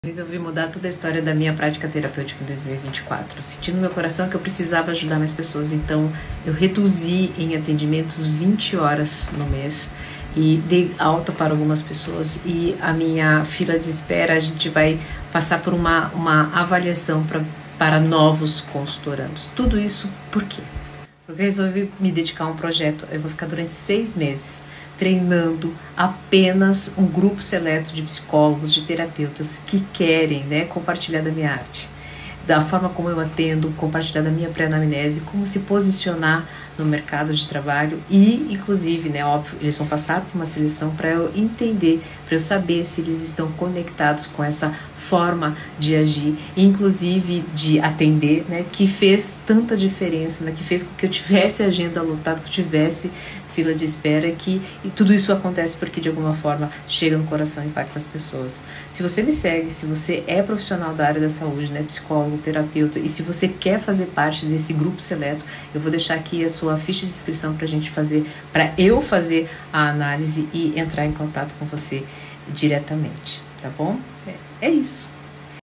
Mentoria Profissional
Para 2024 eu resolvi mudar a minha vida profissional.

Desde que eu entrei no mundo digital, em março de 2020, o meu ticket médio com os atendimentos clínicos aumentou 5x e o meu faturamento bruto anual aumentou 7x.

Porém, há aproximadamente um ano e meio que eu não tenho mais agenda disponível para novos consultorandos (pacientes) e não tenho profissionais de minha confiança para indicar quando as pessoas me procuram por atendimento.

E como resolver este problema de agenda?

A minha solução não foi abrir novos horários, pelo contrário. A minha solução foi a de reduzir o número de horas que atendo mensalmente.

Eu sou apenas uma pessoa e não tenho como atender todos que chegam precisando de processo terapêutico.

Então resolvi que está na hora de ensinar o que faço, ensinar como é a minha prática para identificar o Corpo de Dor e como silenciá-lo, como fazer as pessoas reconhecerem que estão em Cegueira Emocional ou em Paralisia Emocional, para outros psicólogos e terapeutas.

E alinhada com essa minha decisão, eu precisei reduzir em mais de 20h o total de horas de atendimento no mês, para poder me dedicar a um grupo seleto de profissionais que irão ter o meu acompanhamento pelo período de 6 (seis) meses.

Além destes profissionais aprenderem sobre a minha prática clínica, eles também aprenderão a como produzir conteúdo,  posicionamento nas redes sociais, como gerar conexão com as pessoas, e noções básicas de gerenciamento empresarial. O psicólogo e o terapeuta,  são uma empresa, e precisam aprender a se comportar como uma.

E eu quero te ajudar, pois eu já passei por isso.

Inclusive, eu passei da crença que a psicologia não era uma profissão financeiramente viável, para a realidade que a psicologia me deu Liberdade Emocional, Liberdade Profissional e Liberdade Financeira.

Então se você, é Psicólogo ou Terapeuta, deseja fazer uma mudança na sua vida profissional, e consequentemente na sua vida pessoal, eu te convido a fazer parte deste seleto grupo.

Para isso, te convido a preencher o formulário para mais informações de como será essa mentoria.

DONA ANA 💕🌿
Ana Paula da Rocha Lima
Psicóloga, CRP-08/15011